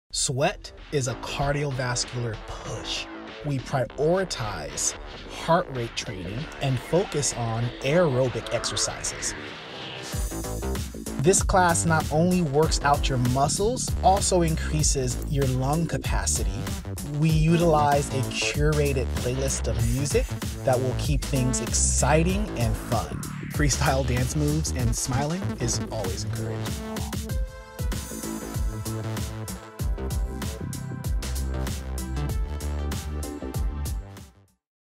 ODC fit SWEAT
Sweat is for cardio lovers! Our instructors guide you through interval training intended to make you sweat and help you push past your perceived boundaries. We combine fitness with dance, kickboxing, calisthenics, and heart-rate training in each class. Equally inspiring as it is challenging, Sweat is a great class for someone who is already active or for those seeking an intermediate level workout. A class that cranks the energy!